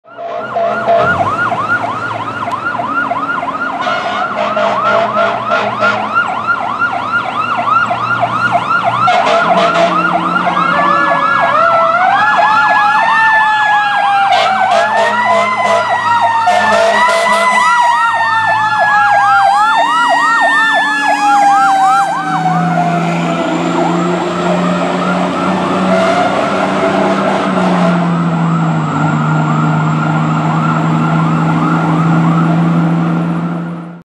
Scranton Fire Department Engine 10 Responding
Scranton Fire Department Engine 10 responding to a carbon monoxide alarm in the City of Scranton, Lackawanna County, PA.